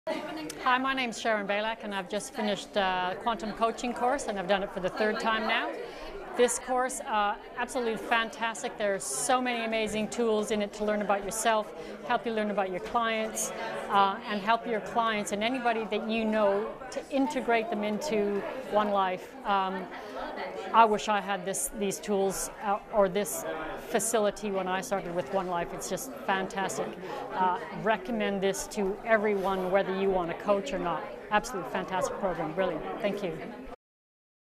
Roy McDonald OneLife Testimonial - Sharon Baylak - Gold Coast
Hi my name is Sharon Baylak and I've just finished the Quantum coaching course and I've done it for the third time now. This course, absolutely fantastic! There are so many amazing tools in it to learn about yourself, how to learn about your clients and help your clients and anybody that you know to integrate them into OneLife. I wish I had these tools or this facility when I started with OneLife. It's just fantastic! I recommend this to everyone whether you want a coach or not. Absolutely fantastic program, brilliant! Thank you.